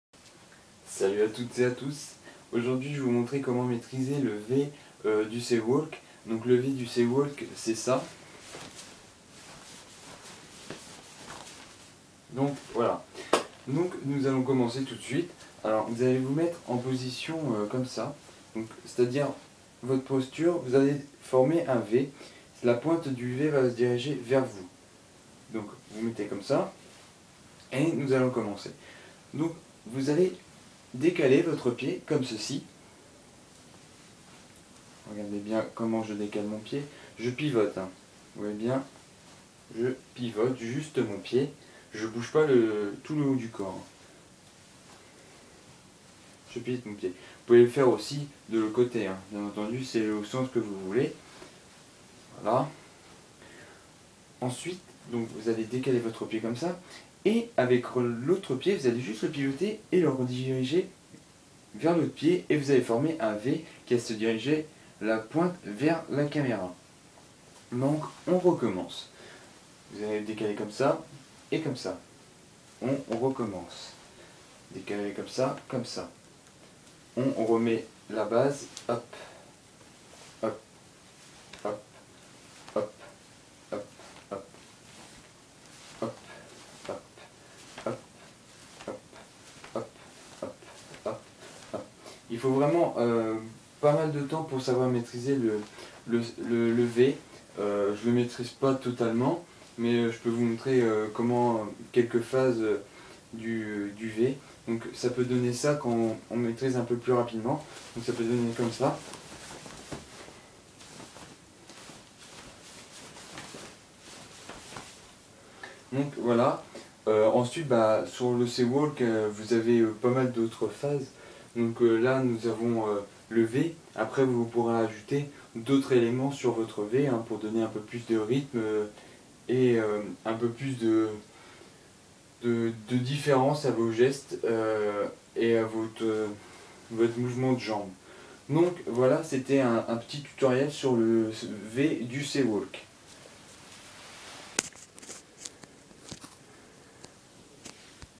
Tutoriel danse - V du C-Walk
Astuce pour danser le V du C-Walk présentée par PratiqueTV